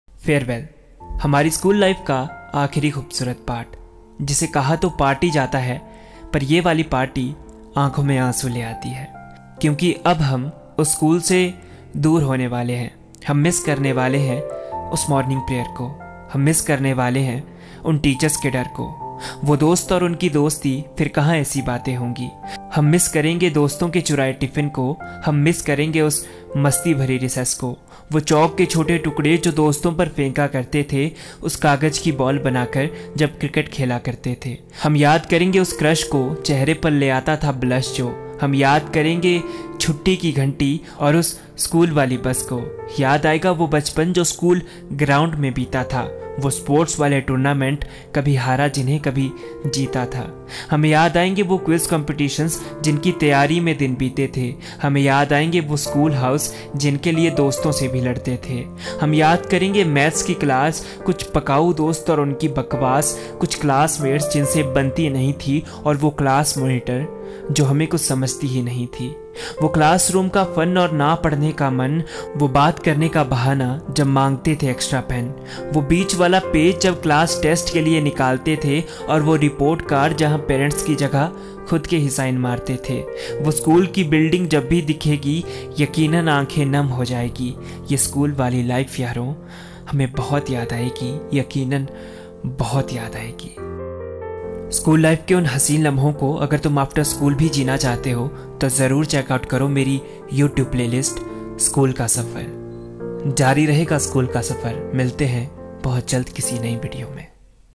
Farewell : An Emotional Poetry On School Life | school life memories | School ka safar ep.40
Listen me on spotify Our new Playlist " school ka safar " is dedicated to all our school time memories
This is a poetic way to Travel all those memories of school life again

I promise this trip will be amazing and enjoyable
Let's enjoy that school time again

Kesi lagi video?